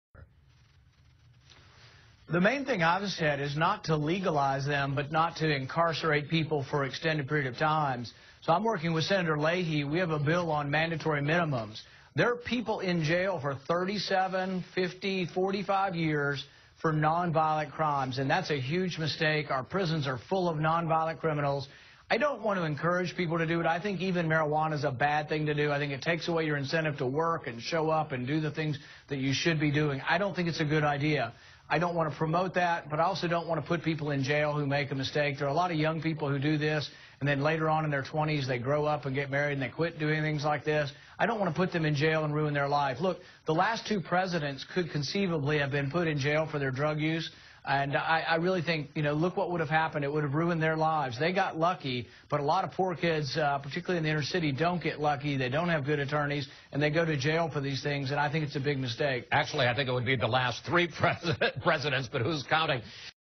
Rand Paul On Marijuana
Rand Paul said he doesn't want to "push" marijuana, but he doesn't want people's lives ruin over a little pot.